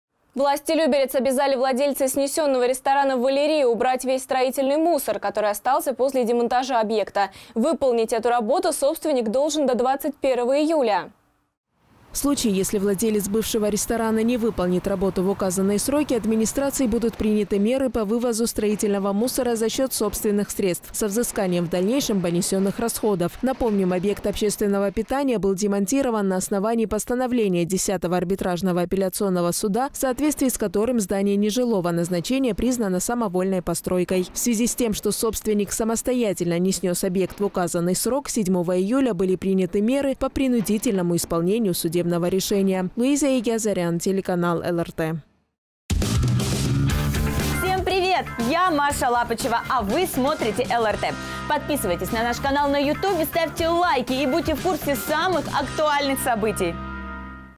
Владельца снесенного ресторана обязали убрать строительный мусор
Власти Люберец обязали владельца снесенного ресторана «Валери»  убрать весь строительный мусор, который остался после демонтажа объекта. Выполнить эту работу собственник должен до 21 июля.

В случае, если владелец бывшего  ресторана  не выполнит работу в указанные сроки, администрацией будут приняты меры по вывозу строительного мусора за счет собственных средств со взысканием в дальнейшем понесенных расходов.

Напомним, объект общественного питания был снесен на основании постановления Десятого арбитражного апелляционного суда, в соответствии с которым здание нежилого назначения признано самовольной постройкой. В связи с тем, что собственник самостоятельно не снес объект в указанный срок, с 7 июля были приняты меры по принудительному исполнению судебного решения.

Луиза Егиазарян